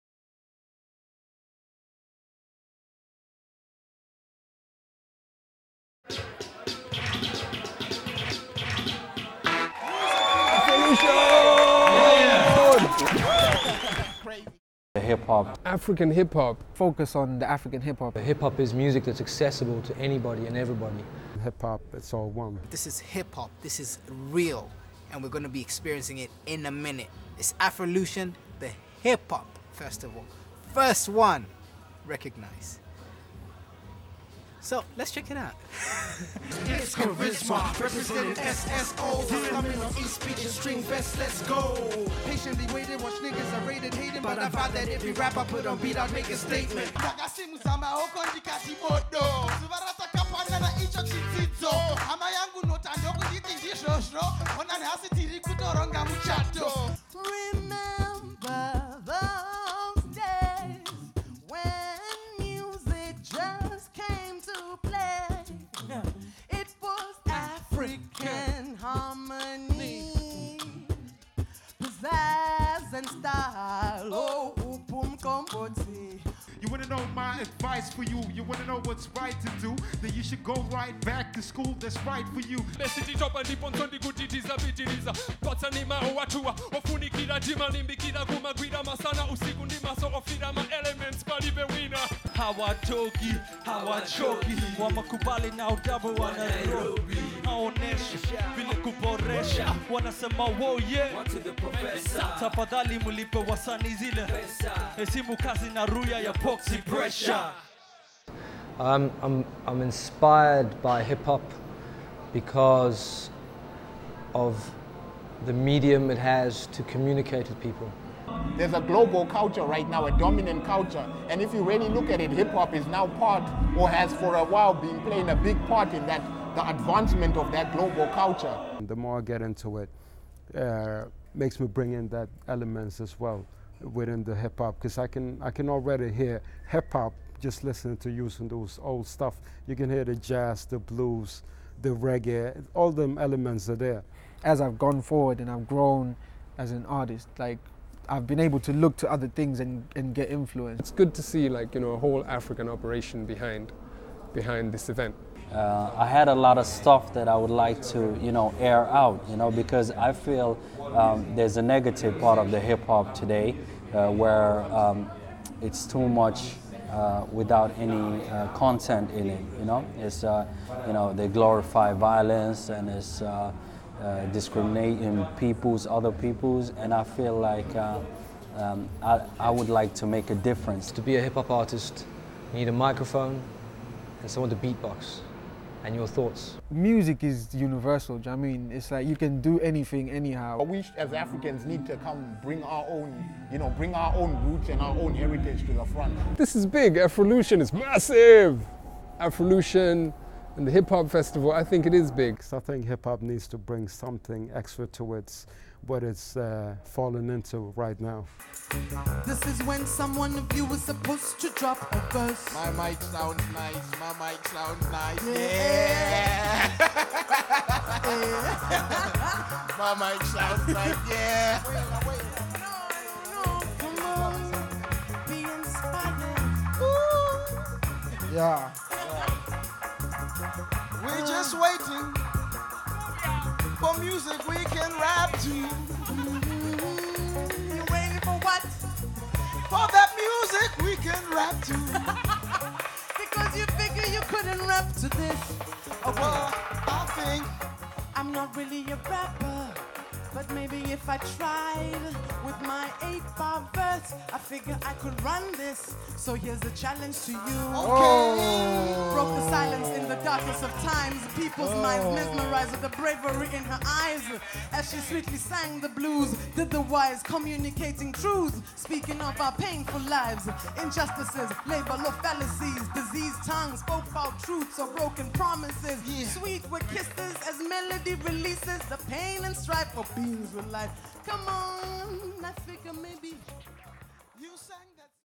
afrolution festival dvd - promo 1
The Afrolution Hip Hop festival brought together African Hip Hop Artists on the same stage for the first time. the DVD of this festival will be available from April 2008 via the Afrolution website.